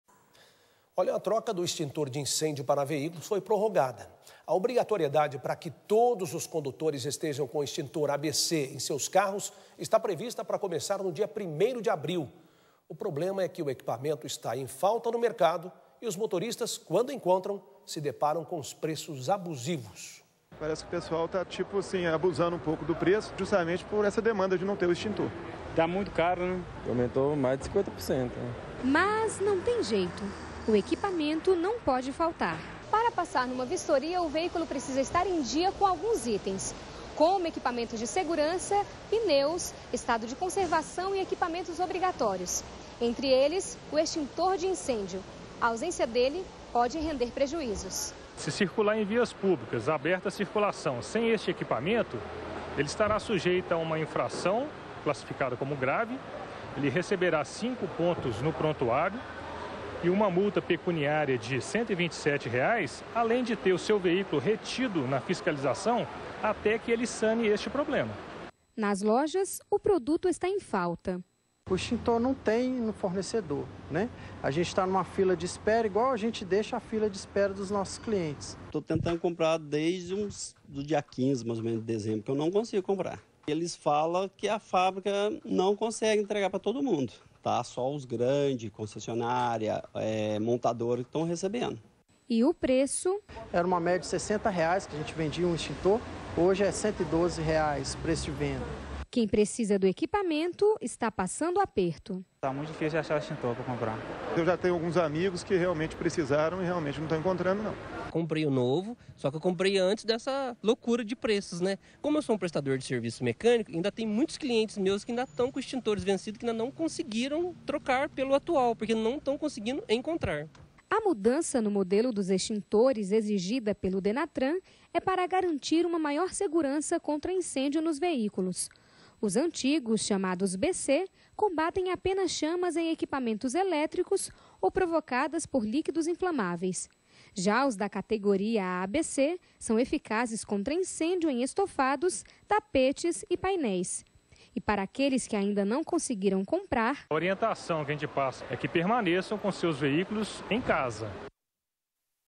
Veja: a saga dos motoristas à procura pelos extintores ABC - CN Notícias
A troca do extintor de incêndio para veículos foi prorrogada. A obrigatoriedade pra que todos os condutores estejam com o extintor ABC em seus carros está prevista pra começar no dia 1º de abril. O problema é que equipamento está em falta no mercado; e os motoristas, quando encontram, se deparam com preços abusivos.
Reportagem de Viviane Eloy e Cláudio Fiuza